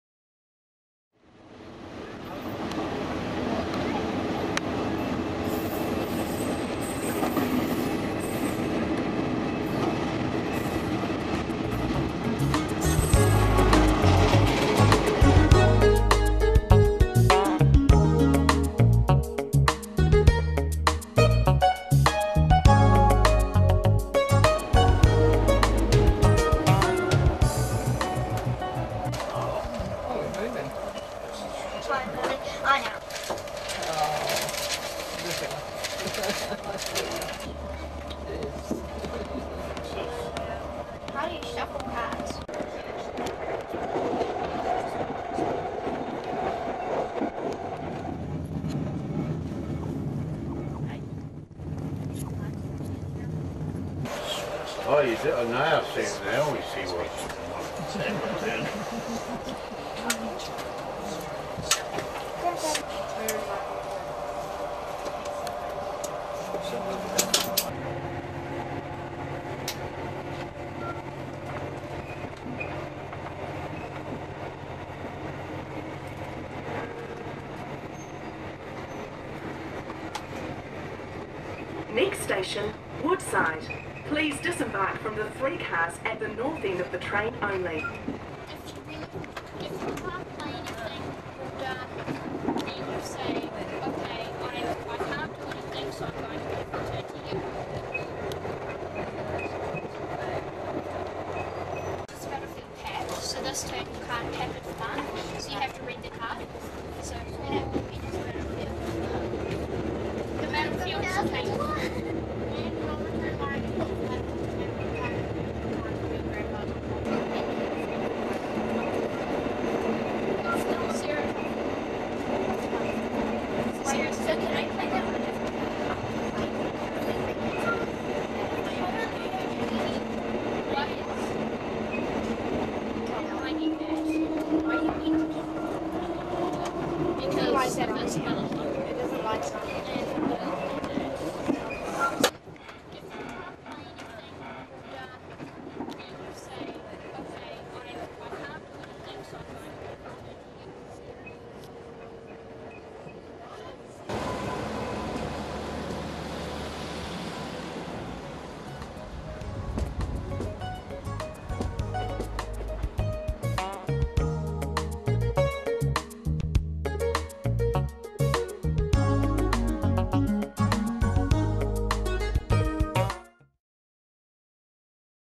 Travelling the Wairarapa Train
3 Days before Christmas 2015. Dft 7160 at the head end as we start off to the capital. A 90 minute journey with 8 minutes in the tunnel, for $18.00. Great value, super train, super journey. Will do again real soon.{Will not play in 16/9. images are 'squashed'. Have tried MANY time to fix it .